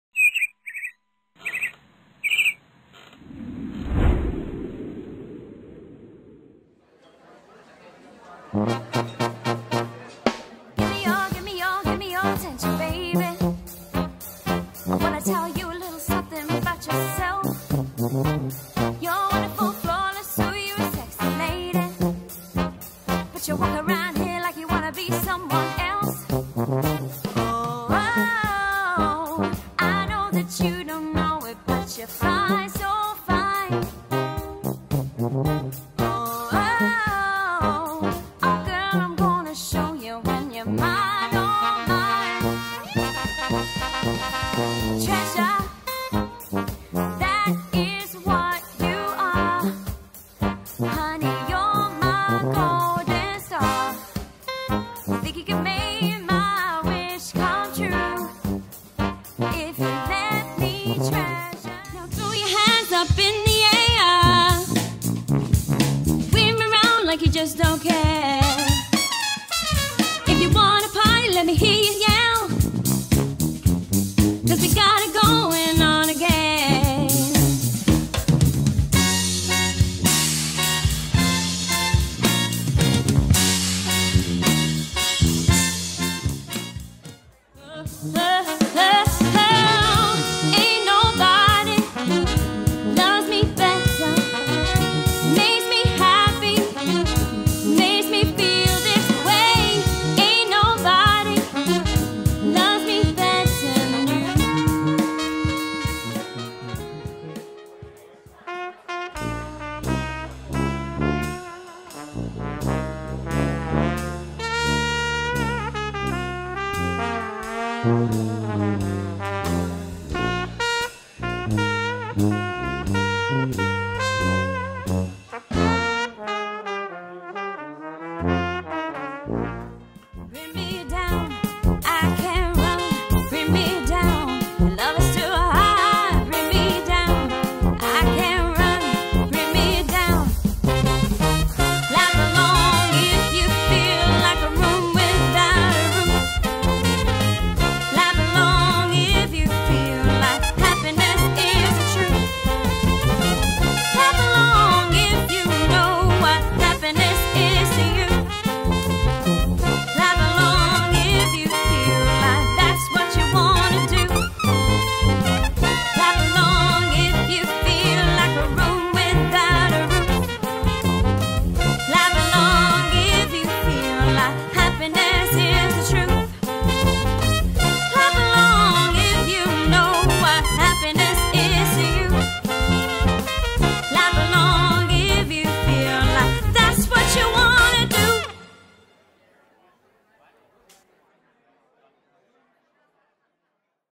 The Mix Street Band for Hire in Surrey from
With vocalist, sousaphone, trombone, trumpet, saxophone and drums, the Mix Street Band fuses the foot tapping feel of a New Orleans street band with some of the most popular chart hits from the 20th and 21st century. Bringing together musicians with a wealth of experience from a wide range of genres, Mix Street Band is cooking on gas, and their sole mission is to entertain!